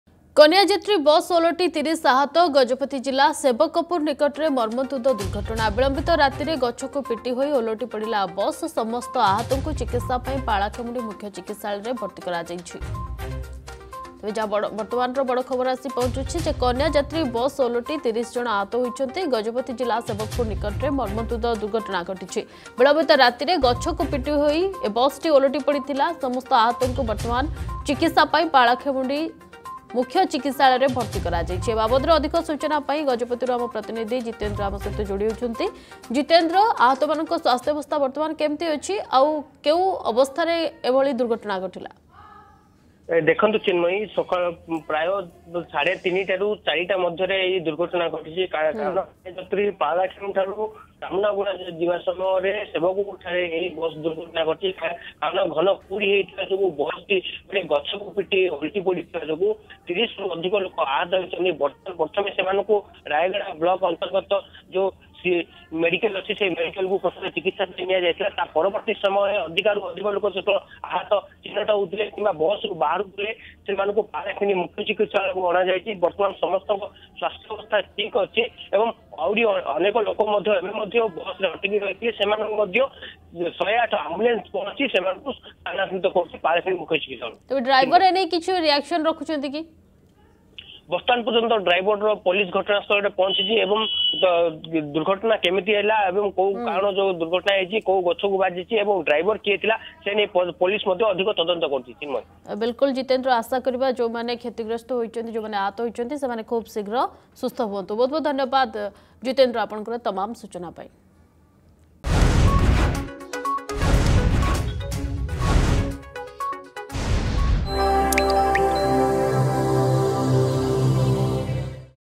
କନ୍ୟା ଯାତ୍ରୀ ବସ ଓଲଟି ୩୦ ଆହତ ।
କନ୍ୟା ଯାତ୍ରୀ ବସ ଓଲଟି ୩୦ ଆହତ । ଗଜପତି ଜିଲ୍ଲା ସେବକପୁର ନିକଟରେ ମର୍ମନ୍ତୁଦ ଦୁର୍ଘଟଣା । ବିଳମ୍ବିତ ରାତ୍ରିରେ ଗଛକୁ ପିଟି ହୋଇ ଓଲଟି ପଡିଲା ବସ୍ ।